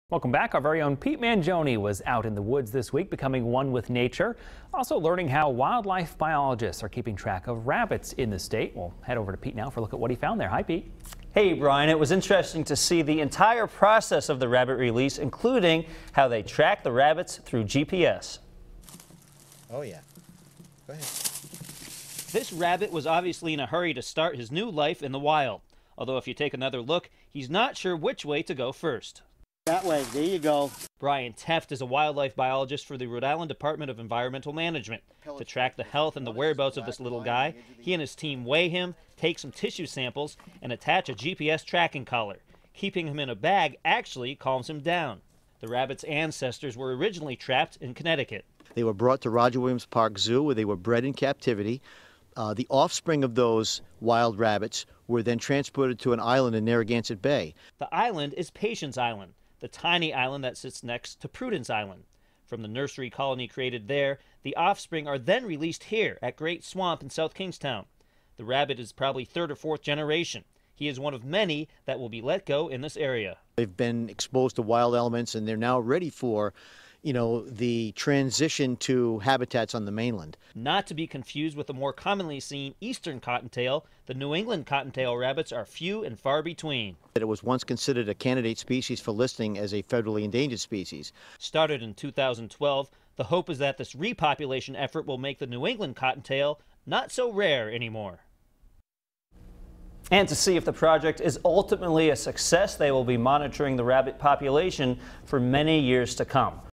A Rabbit's Journey: Looking to start a new life
Wildlife biologists for the Rhode Island Department of Environmental Management want to track the whereabouts of rabbits, making sure they are safe.